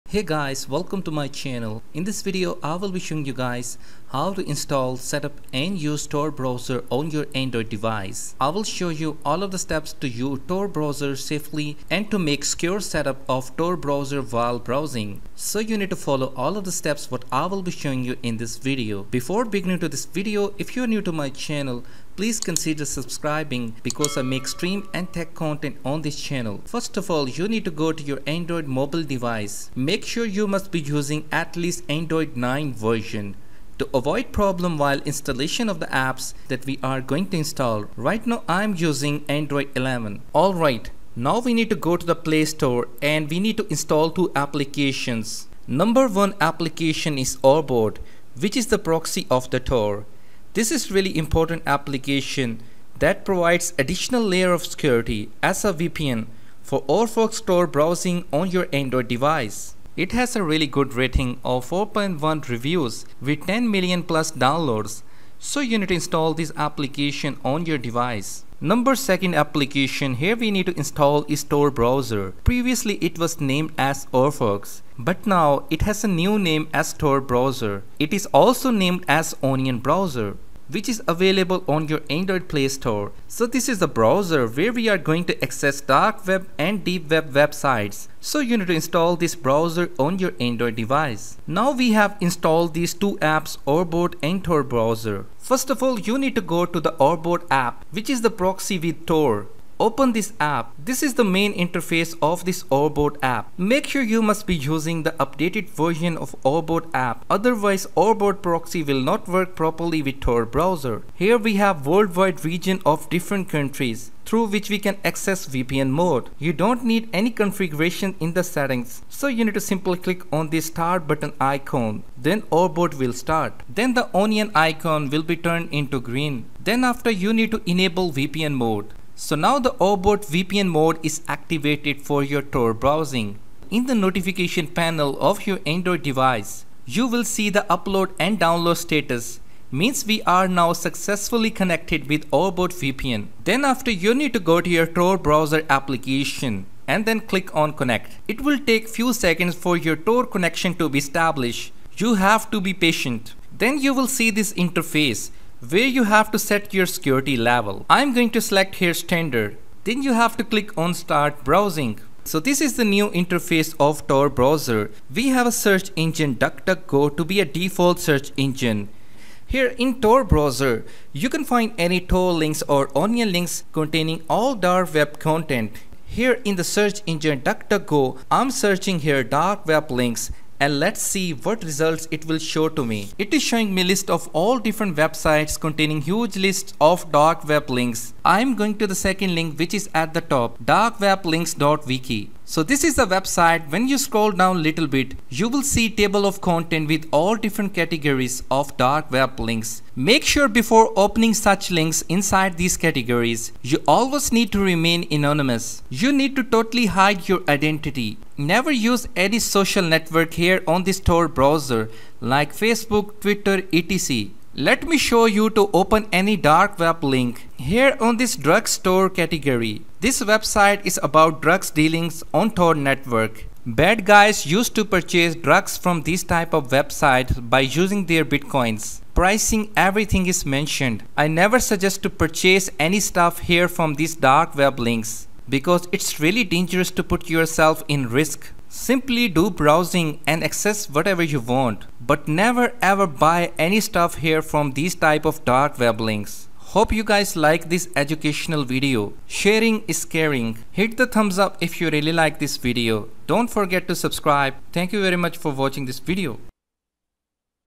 How to Use Tor Browser Safely on Android 2024
Hey Guys Welcome back to another video I will show you in this video how to use tor browser android  and will show you the detailed  demonstration for the setup of Tor Browser  Orfox on android mobile .